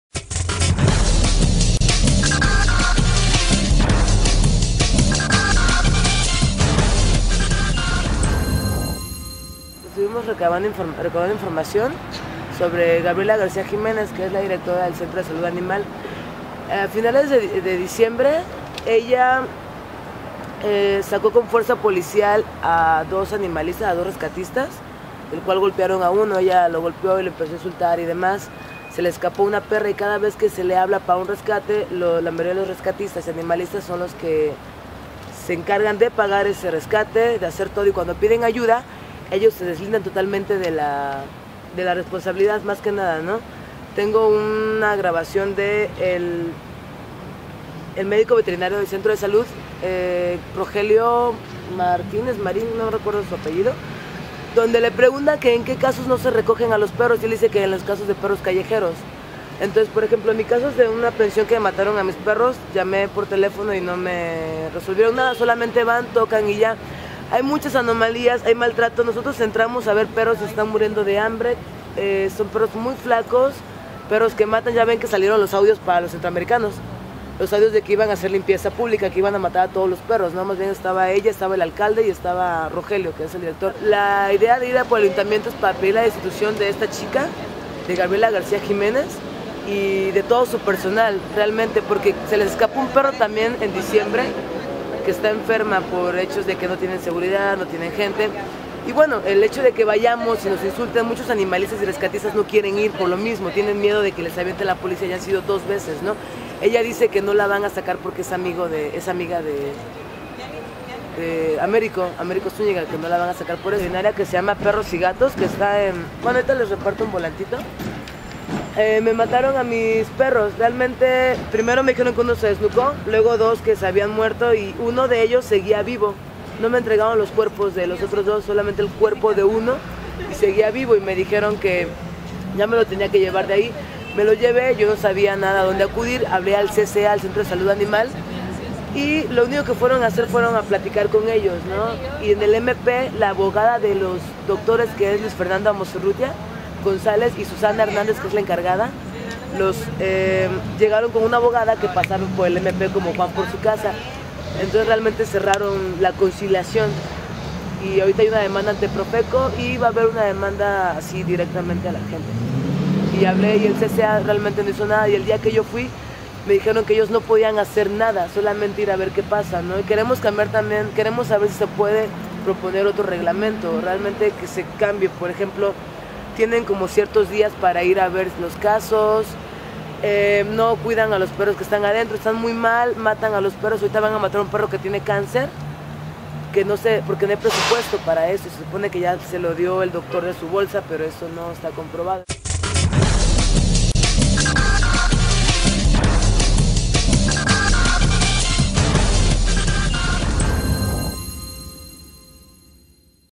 Continúan las denuncias contra directora del Centro de Salud Animal en Xalapa
Xalapa, Ver.- Nuevamente la directora del Centro de Salud Animal en Xalapa, Gabriela García, fue denunciada por una ciudadana que afirma tener pruebas de maltrato e ineficacia de ese centro, asimismo pide la destitución la cual ve difícil debido a que la directora se dice "inamovible" por la relación de amistad que tiene con el alcalde, Américo Zúñiga.